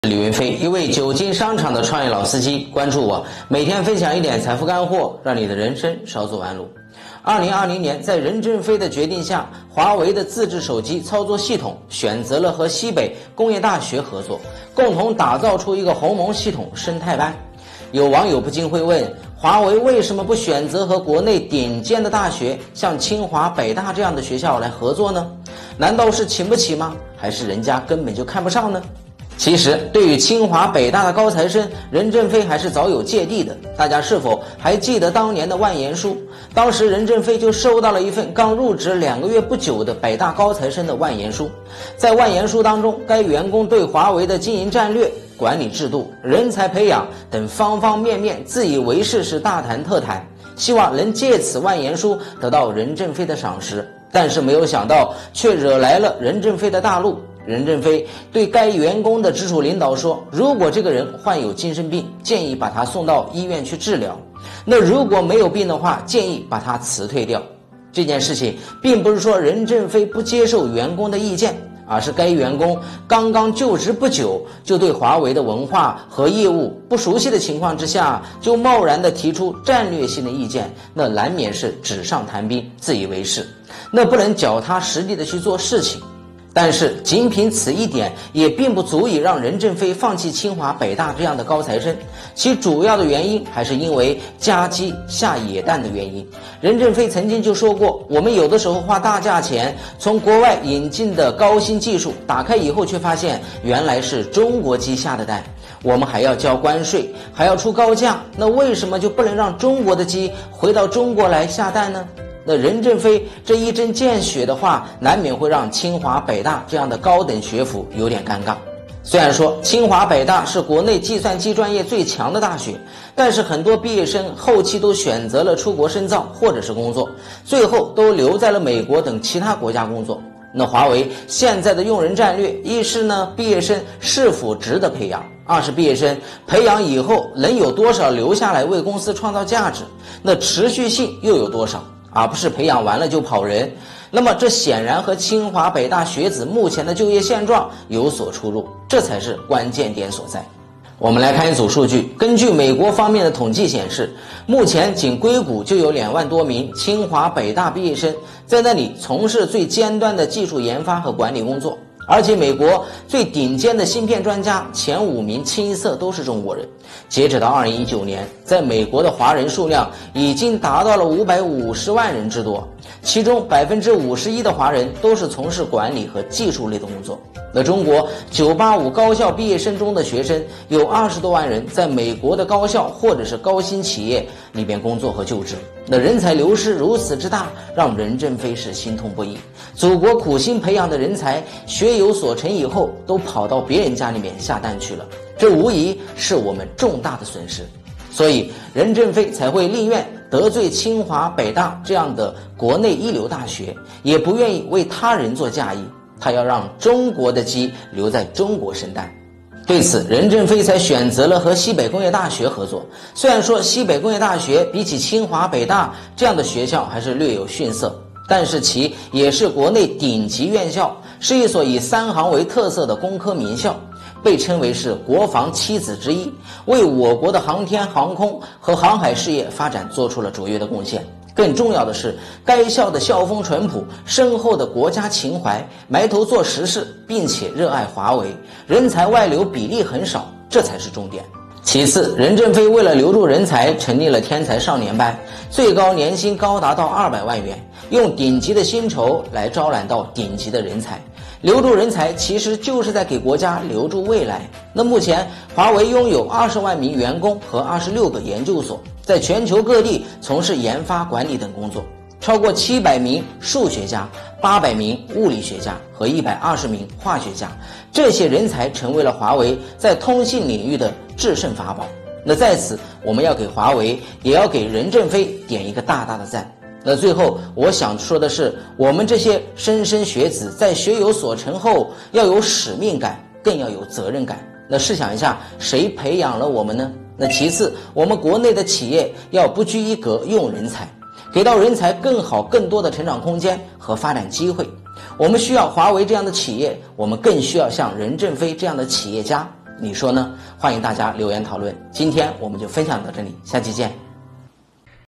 华为宁愿选择“西工大”，也不看清华北大一眼，任正非说到点子上
华为宁愿选择“西工大”，也不看清华北大一眼，任正非说到点子上,本视频由李云飞Afey原创提供,60次播放,好看视频是由百度团队打造的集内涵和颜值于一身的专业短视频聚合平台